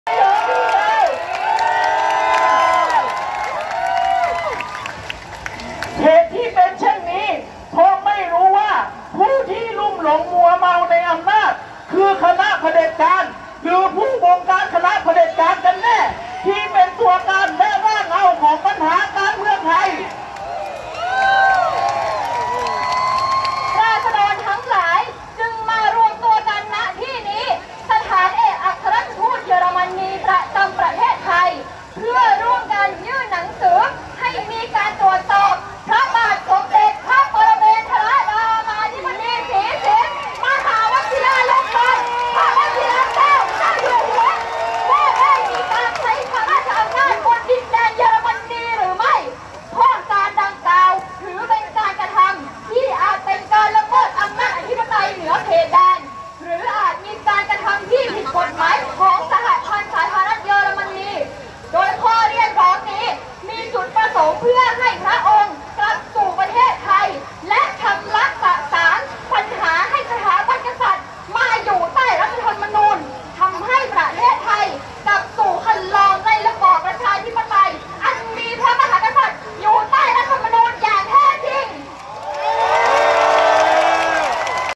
แถลงการณ์จากราษฎร ณ สถานทูตเยอรมันฯ | 26 ต.ค. 63
26 ต.ค. 63 เวลา 21.50 น. ณ สถานเอกอัครราชทูตสหพันธ์สาธารณรัฐเยอรมนี กรุงเทพฯ ผู้ชุมนุมกลุ่มคณะราษฎรยืนอ่านแถลงการณ์เรื่อง 'การเดินขบวนมายังสถานทูตสหพันธ์สาธารณรัฐเยอรมนี' หลังจากเดินขบวนจากบริเวณแยกสามย่าน มาจนถึงสถานทูตเยอรมันฯ เมื่อช่วงเย็นของวันที่ 26 ต.ค.